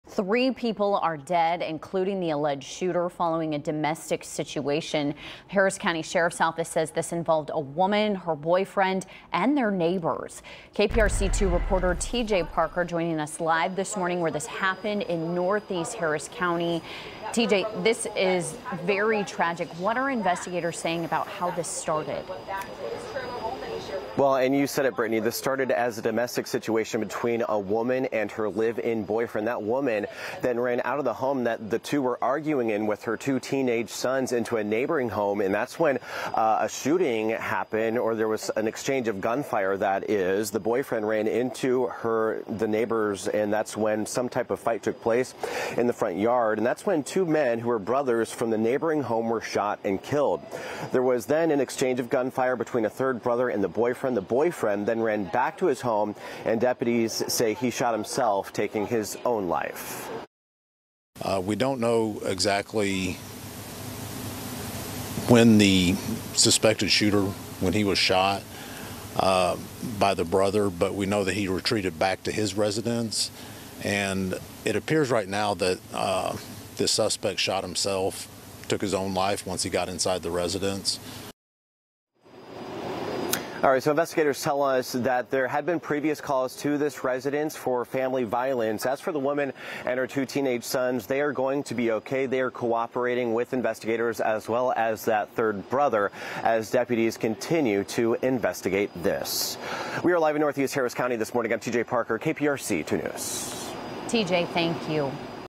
Three people dead in apparent murder-suicide in NE Harris County
Investigators with Harris County Sheriff's Office say they believe the shooting stems from a domestic violence incident.